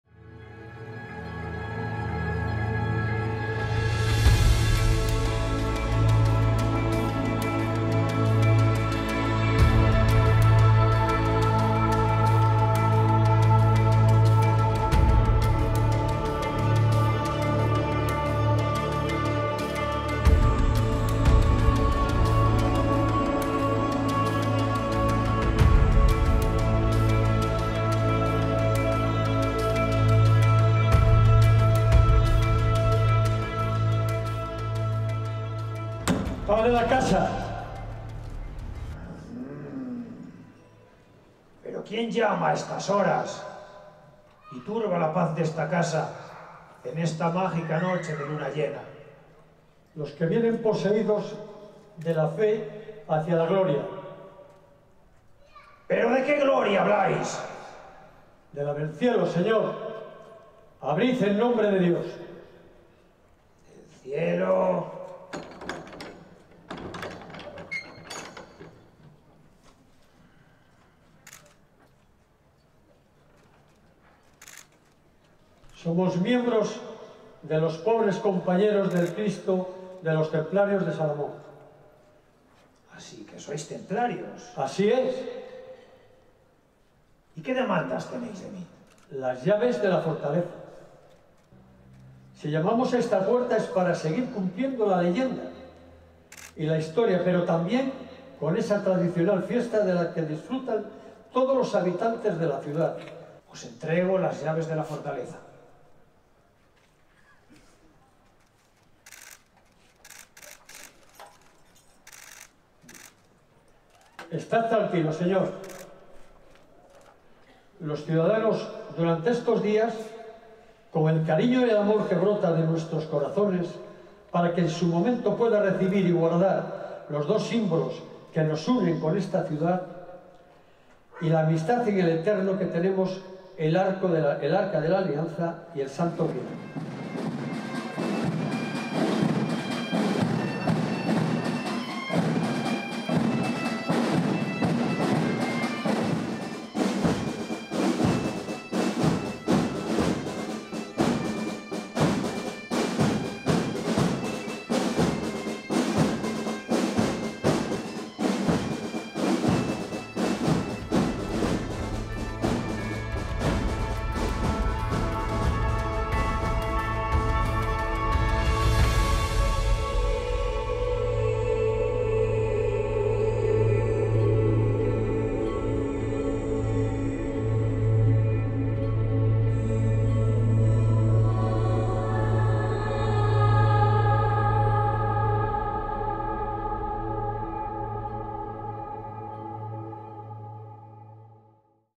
Los caballeros del Grial abren las puertas del Castillo para la celebración de la Noche Templaria
Con la celebración de la Quiebra de la Luna Templaria se da paso a las principales actividades programadas para la Noche Templaria. Los caballeros del Temple solicitan a los regidores de la ciudad las llaves de la fortaleza de Ponferrada para dar paso a los actos que se celebrarán hasta el próximo domingo 7 de julio.